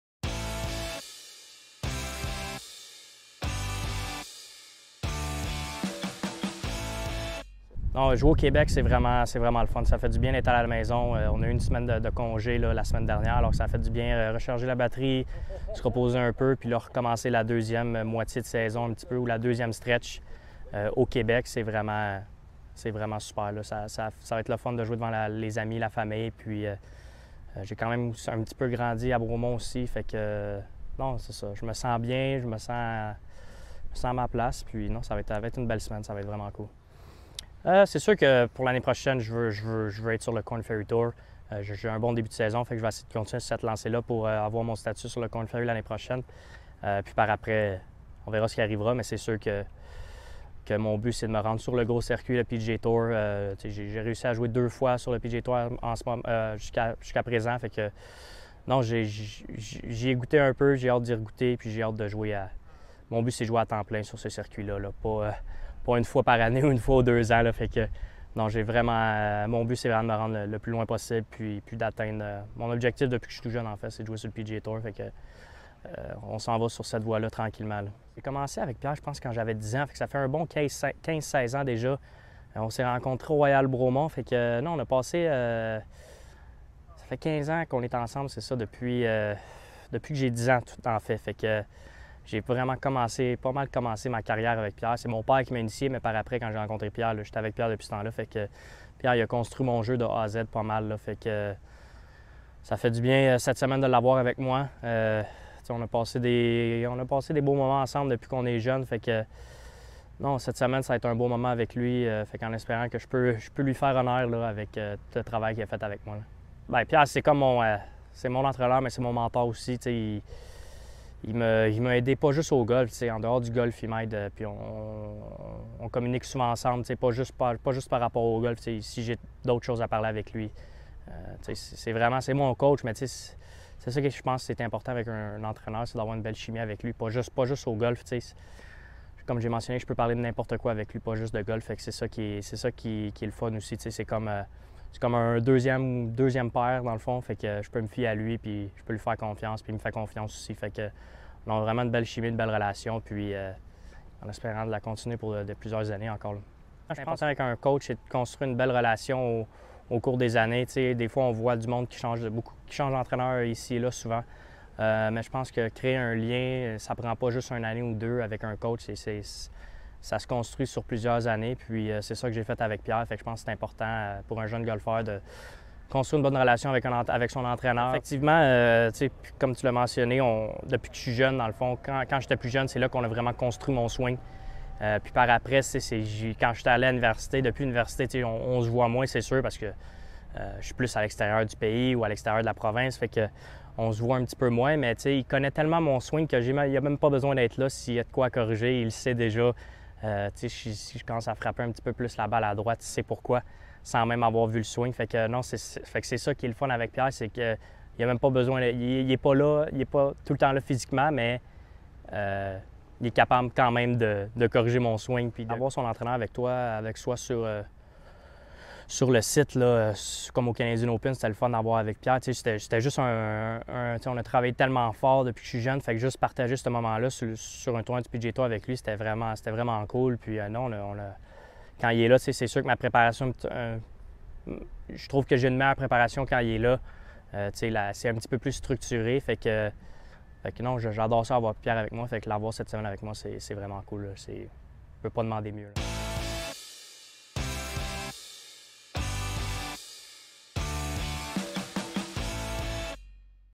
L'impact de Golf Québec | Entrevue avec Étienne Papineau
Nous vous présentons une entrevue réalisée cet été avec Étienne Papineau lors de l'Omnium du Québec.⛳ Il nous parle entre autres de l'impact que son entraîneur, Pierre Dugas, a eu sur son développement vers les circuits professionnels! Vous verrez également qu'en juillet, Étienne avait comme objectif d'atteindre le circuit Korn Ferry pour la saison prochaine, chose qu'il a accompli!👏